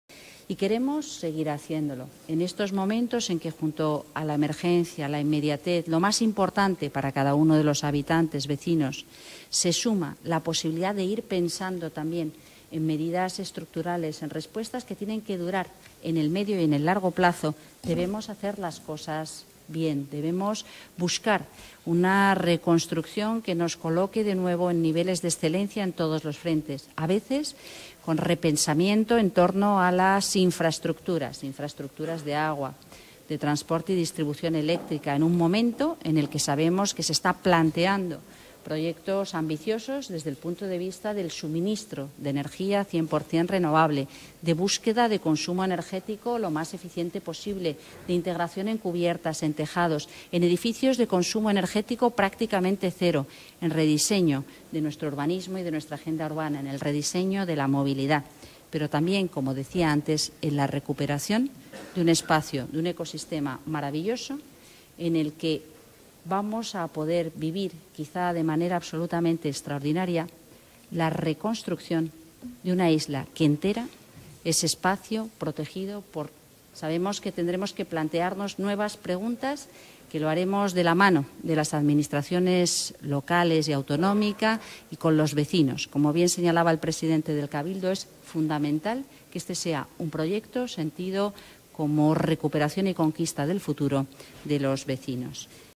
Ribera aboga por "repensar" La Palma para el futuro en una reconstrucción de excelencia
La vicepresidenta tercera y ministra para la Transición Ecológica y Reto Demográfico, Teresa Ribera, ha garantizado el apoyo del Gobierno "siempre que se pueda ayudar" a los habitantes de La Palma y ha animado a "repensar" la isla para acometer la reconstrucción y recuperar la "excelencia" a medio y largo plazo.

(Fuente: ACFIPRESS ACFIPRESS)